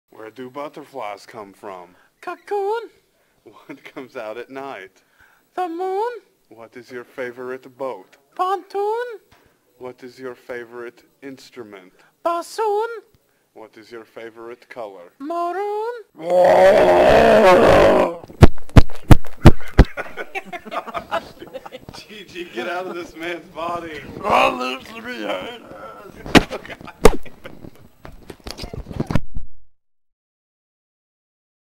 Coccoon Guy
BEHOLD!  It is coccoon guy.  He wants nothing but to inform ou of what rhymes with coccoon.  Yes, that's a GG Allin impersanation at the end.